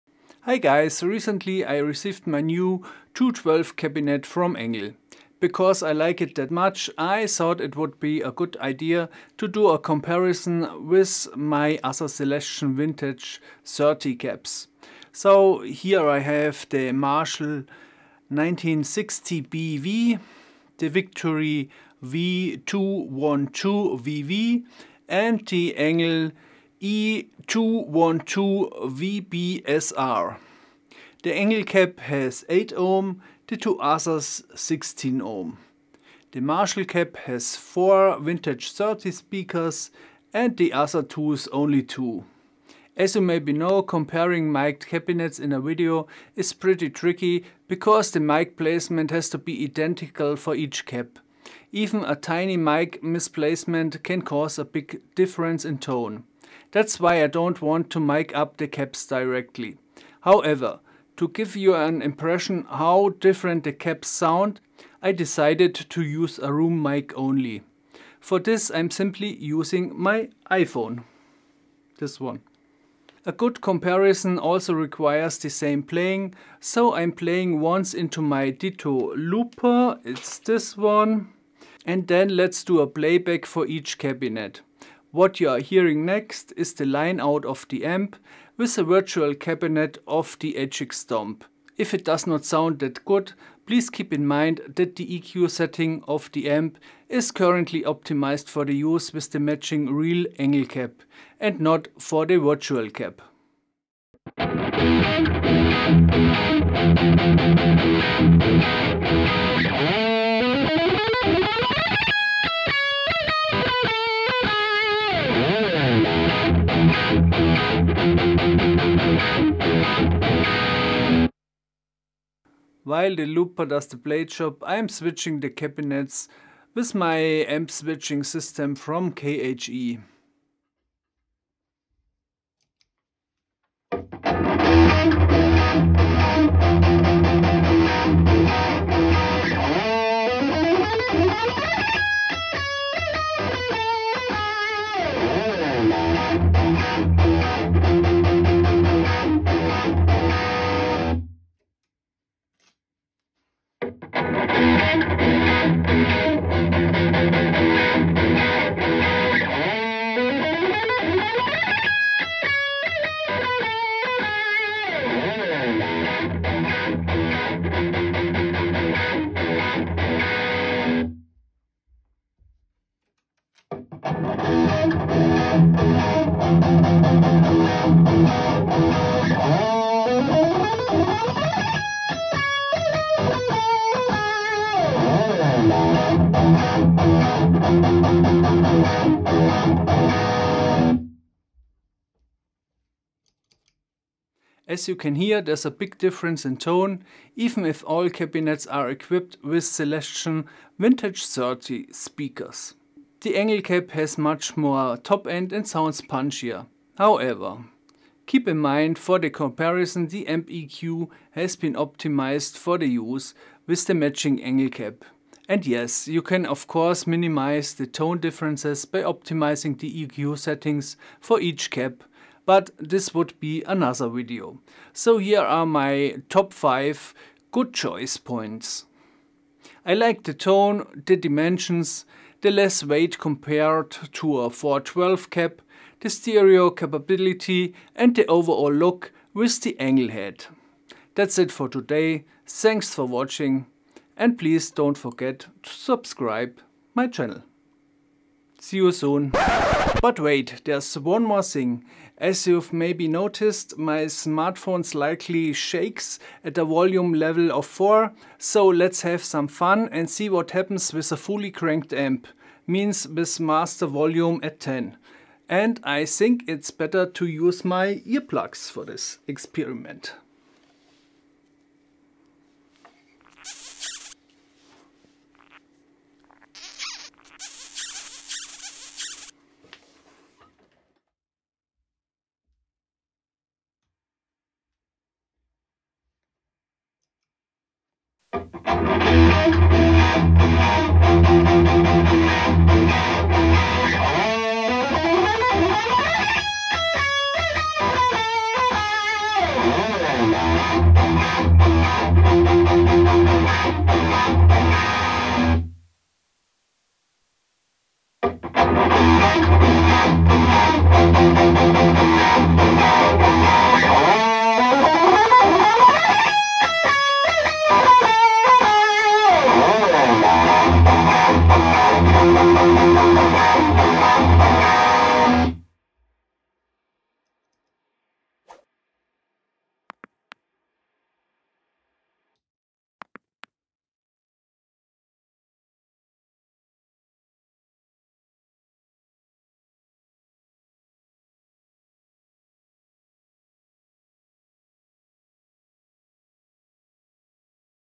Engl, Victory, Marshall cabinet shootout!
Short heavy rock sound comparison of my Marshall 1960BV, Victory V212VV and Engl E212VB cabinets. You'll also hear how a fully cranked Engl amp sounds - have fun!

0:00 Intro
1:43 Playing
2:12 Shootout
3:14 Conclusion
4:09 Fully cranked Engl Fireball 25